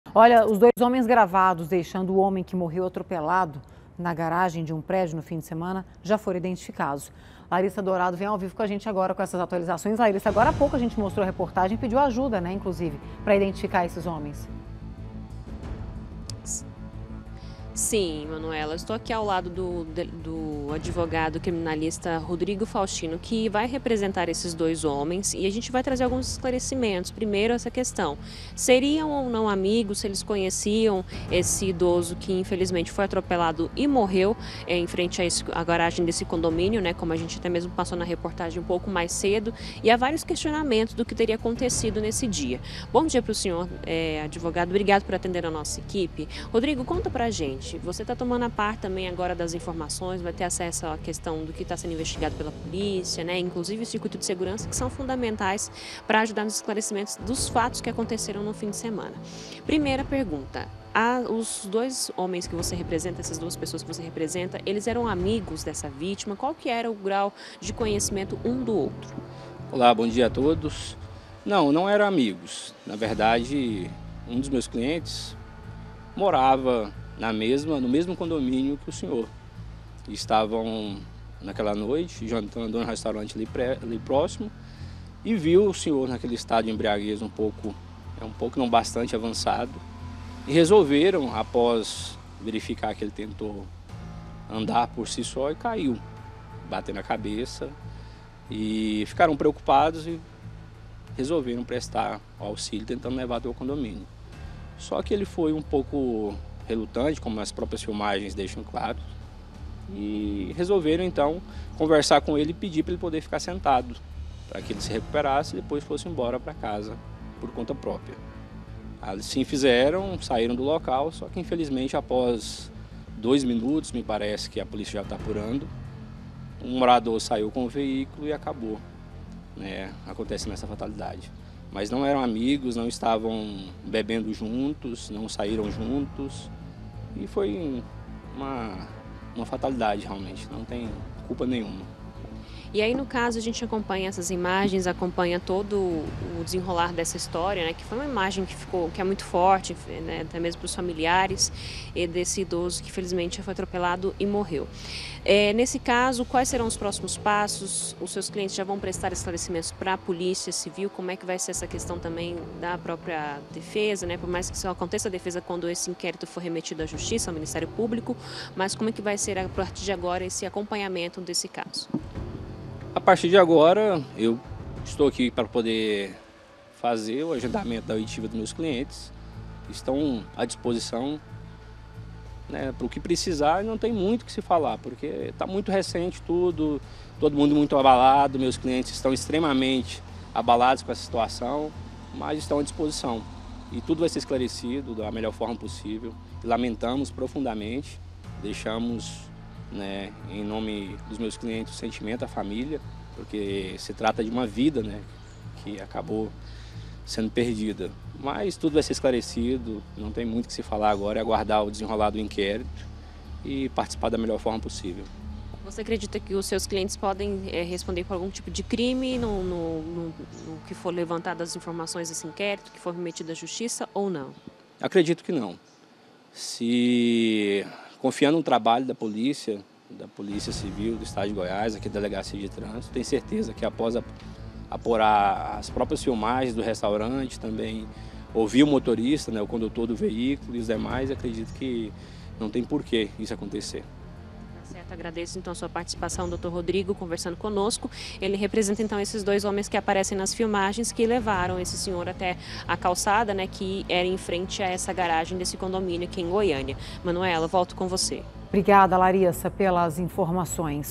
MORTE POR ATROPELAMENTO: IDENTIFICADOS HOMENS QUE APARECEM NO VÍDEO
OS DOIS HOMENS GRAVADOS DEIXANDO O HOMEM QUE MORREU ATROPELADO NA GARAGEM DE UM PRÉDIO NO FIM DE SEMANA JÁ FORAM IDENTIFICADOS. A LARISSA DOURADO CHEGA AGORA COM AS ATUALIZAÇÕES DESSE CASO.